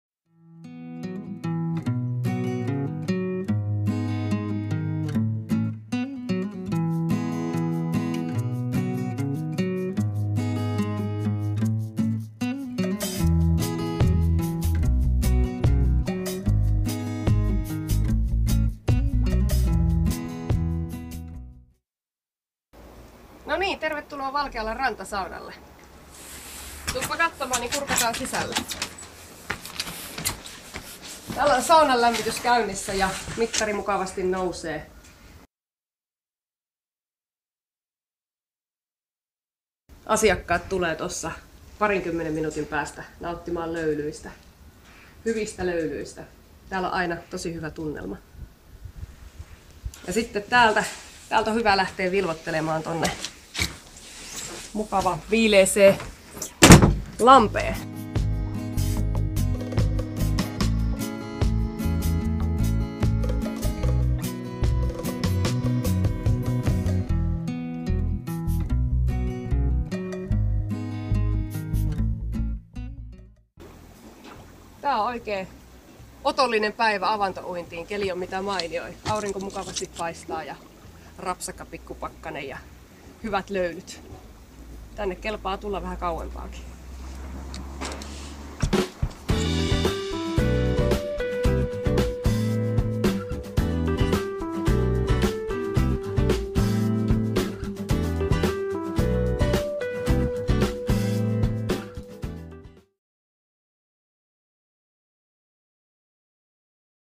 Valkealan rantasauna
Oletko kokeillut avantouintia? Valkealatalolla siihen on erinomainen mahdollisuus! Liikunnanohjaajamme Johanna Immonen esittelee videolla rantasaunaa. Tervetuloa vanhat konkarit ja uudet kävijät!